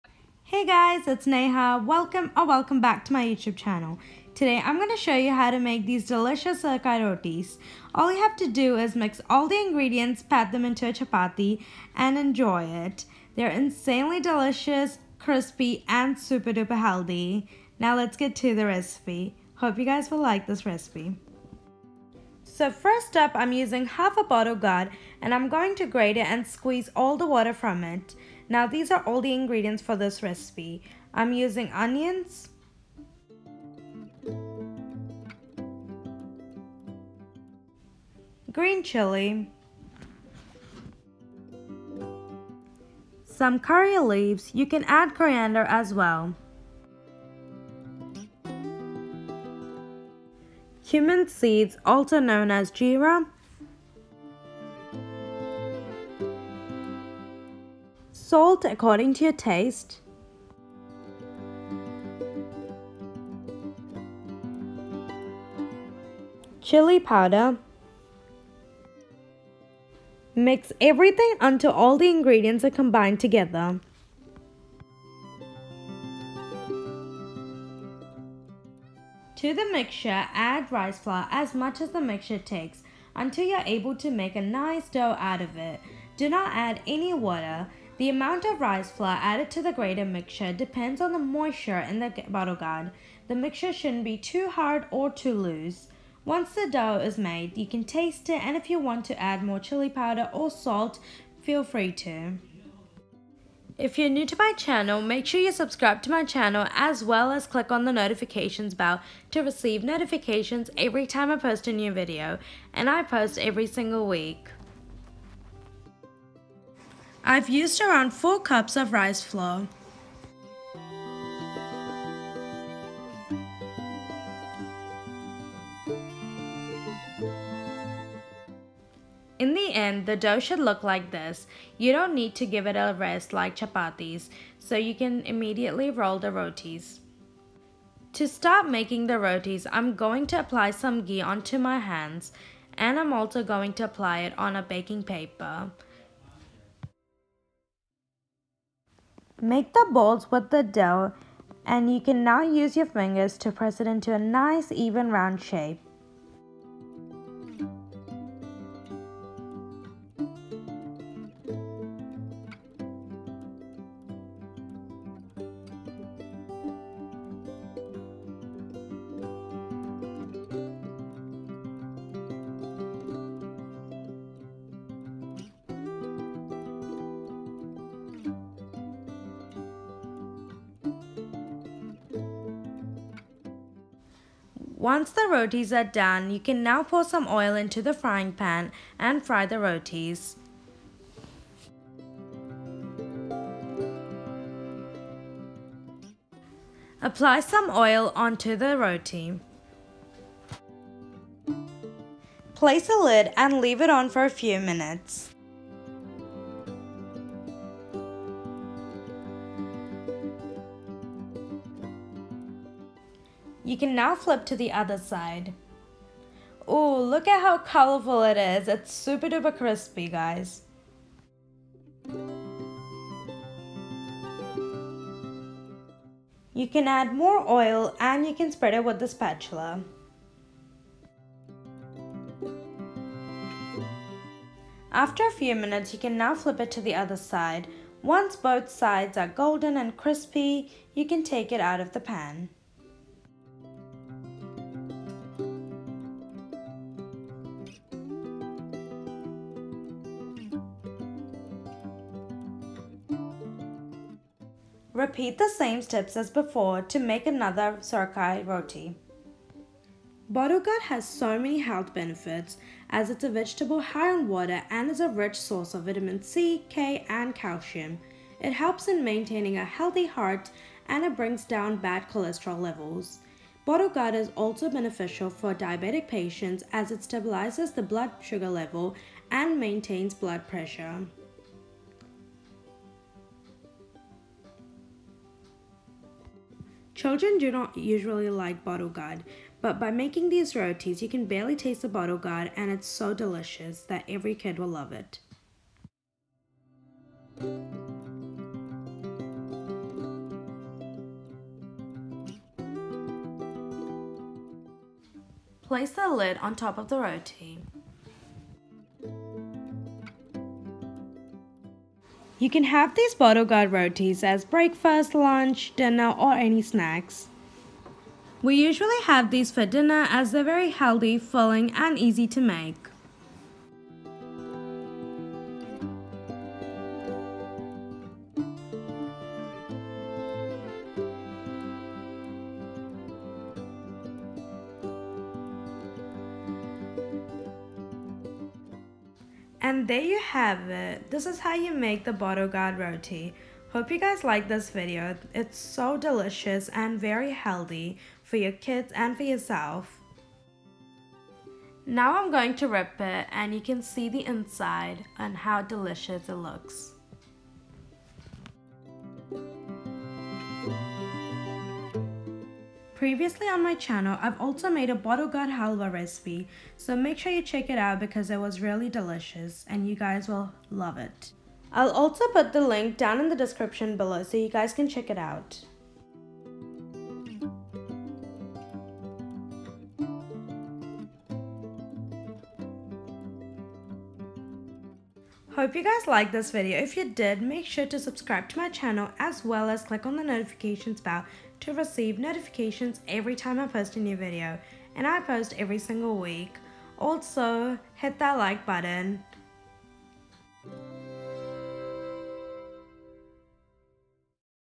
Bottlegourd Roti | Sorakaya Appalu | Sorakaya Appachi | Bottleguard Snacks
Bottleguard Roti | Sorakaya Appalu | Sorakaya Appachi  | Bottleguard Snacks

Ingredients:
Bottleguard
Onions
Chilli
Curry Leaves
Cumin seeds
Salt
Chilli Powder
Rice Flour
Oil